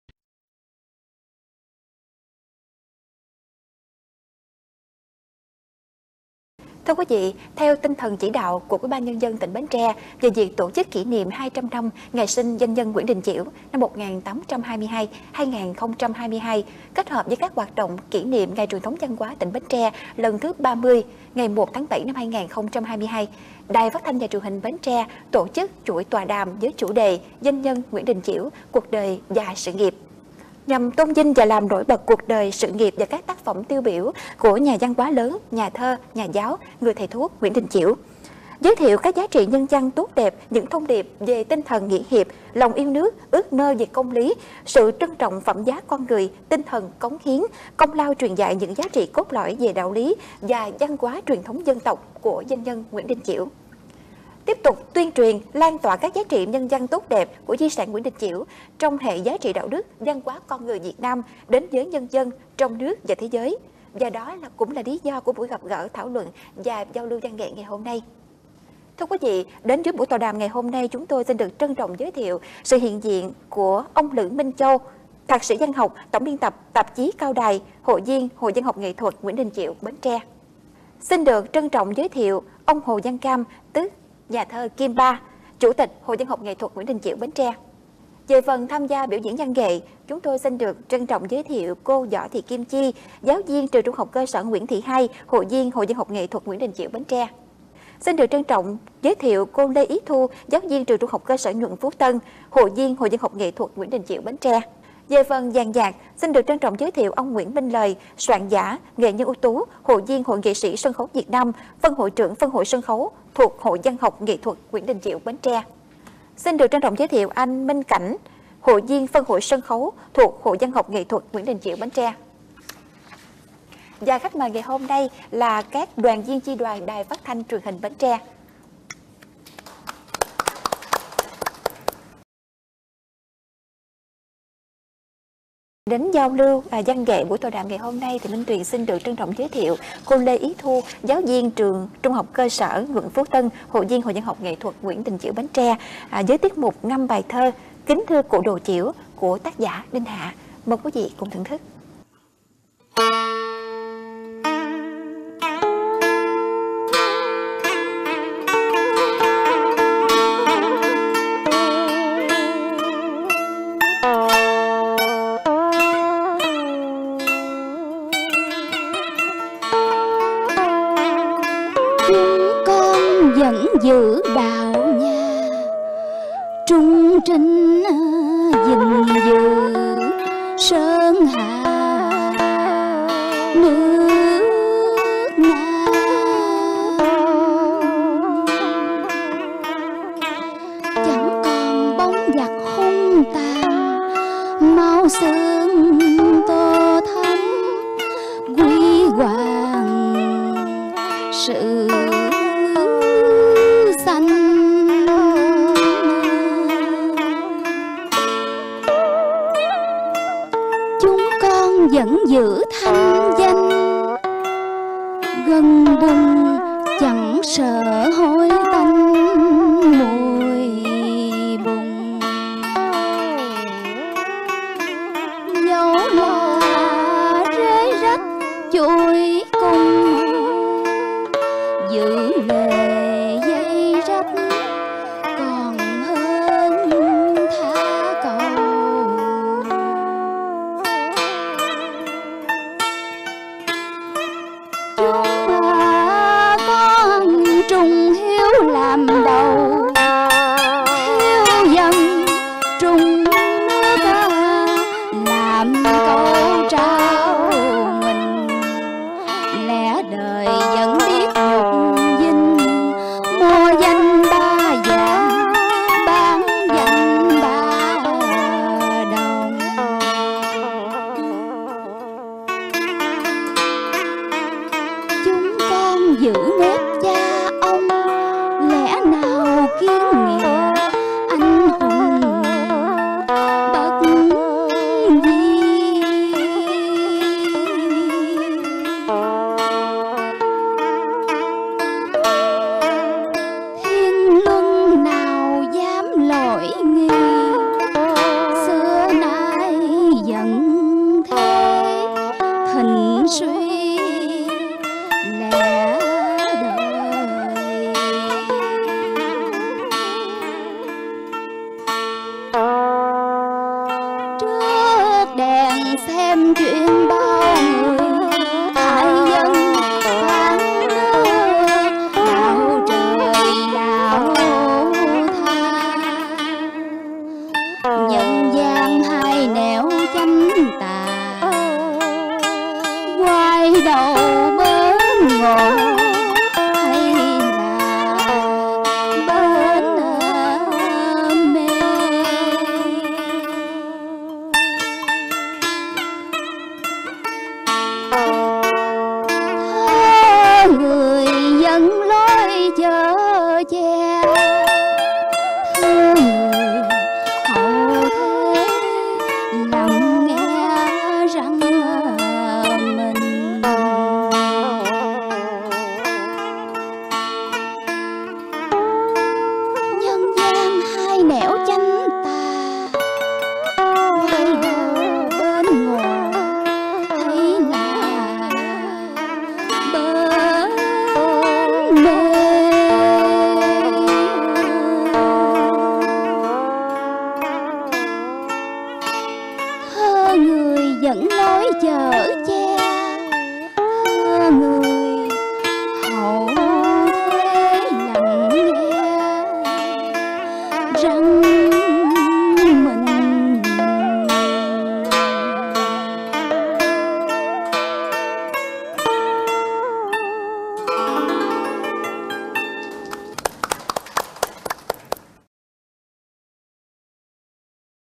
Kính thưa Cụ Đồ Chiểu | Đinh Hạ | Ý Thu | Tọa đàm Cuộc đời và Sự nghiệp Danh nhân Nguyễn Đình Chiểu
Trích chương trình tọa đàm & văn nghệ "Cuộc đời và Sự nghiệp Danh nhân Nguyễn Đình Chiểu"

Bài thơ: Kính thưa Cụ Đồ Chiểu
Tác giả: Đinh Hạ
Diễn Ngâm: Ý Thu